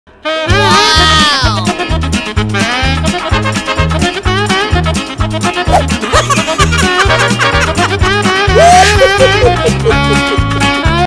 Laughing Through the Wind A Motorcycle Skirt Fiasco 🤣
In this video, an incredibly beautiful girl is riding on a motorcycle when her skirt unexpectedly flies up. Despite the embarrassing situation, she handles it with grace and composure, and manages to laugh it off. Don't miss out on this entertaining clip!